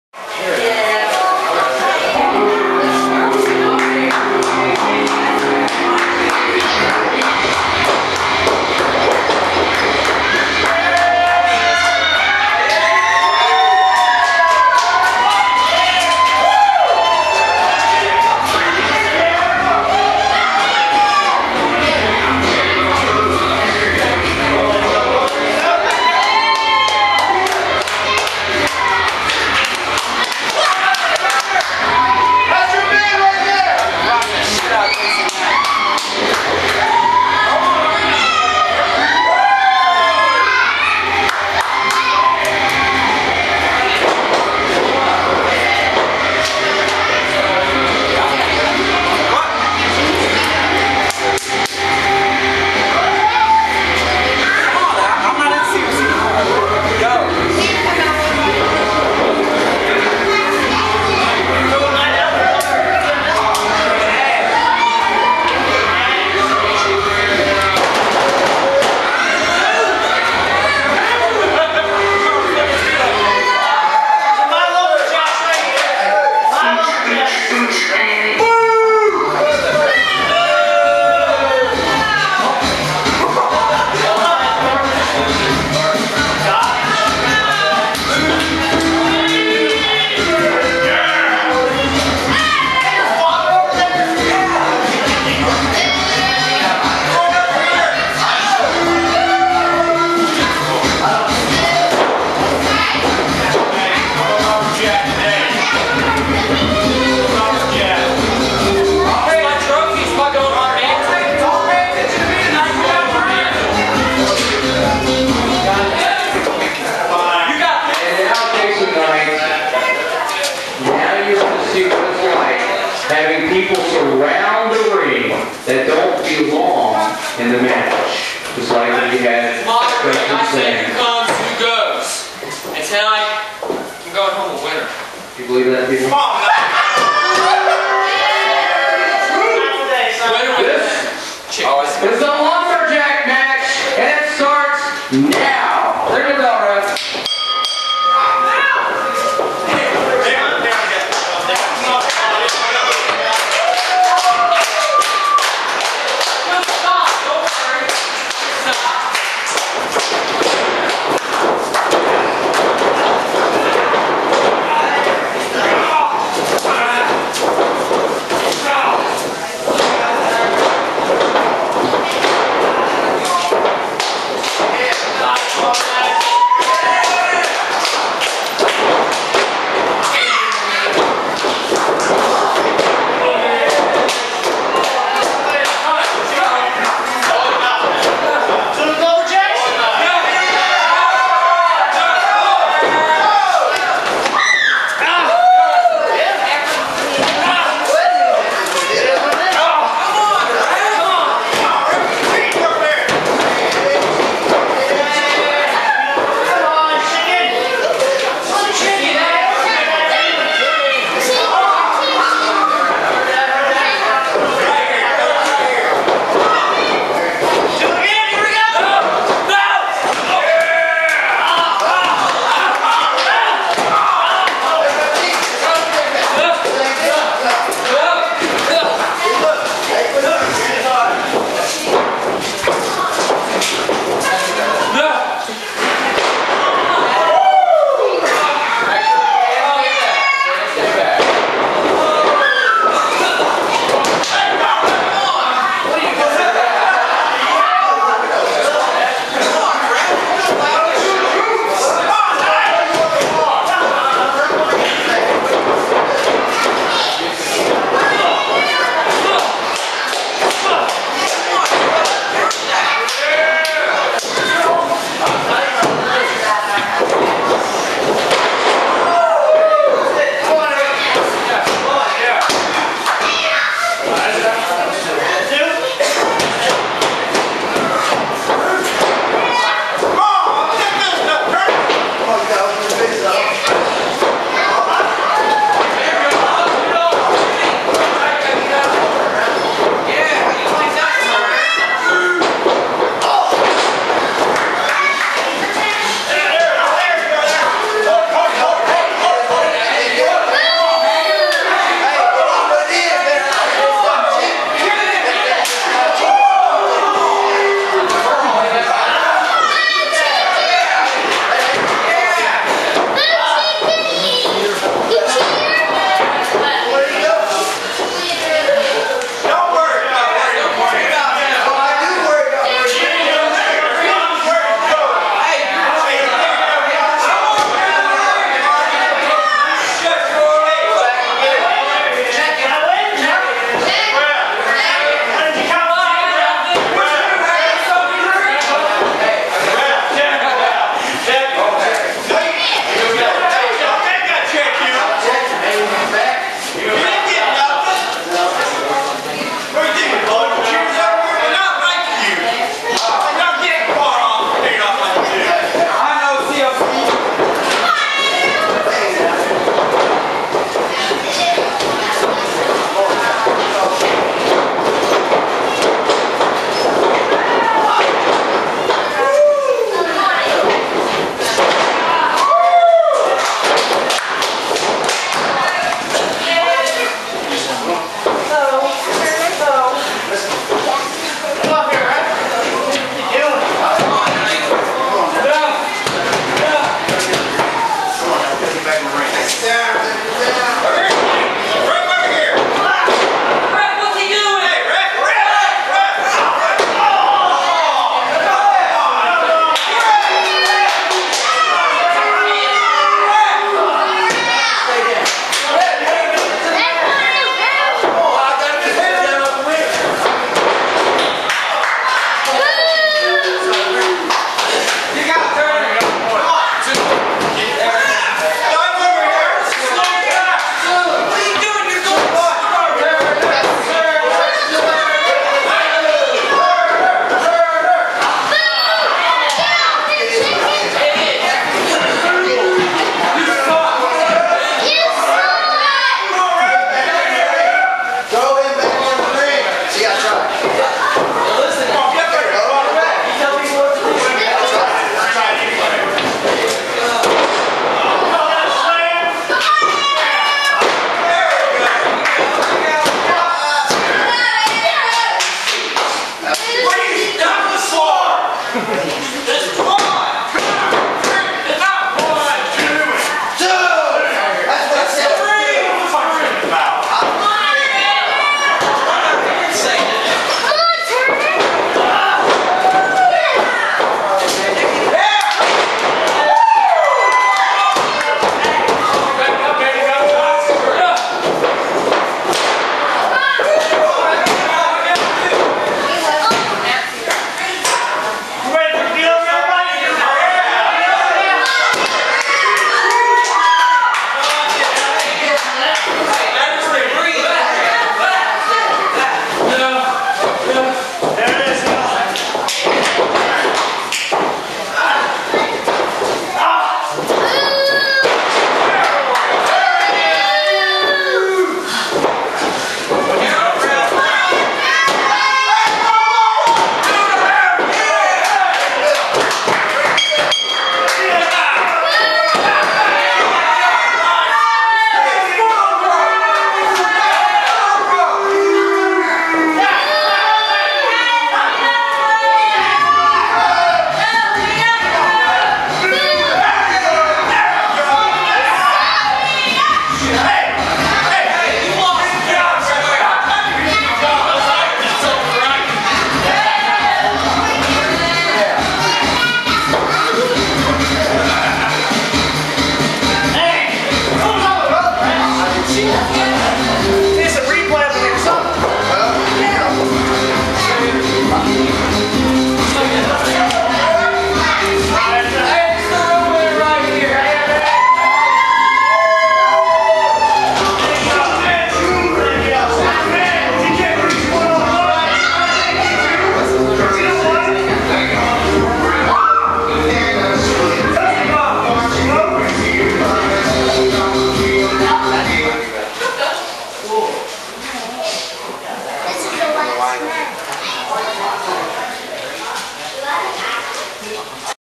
Jason Knight vs Andrew Turner - CWO Lumberjack Match
Director of Authority Frank Hughes had enough of Jason Knight's monopolizing antics and placed him in a lumberjack match against Andrew Turner.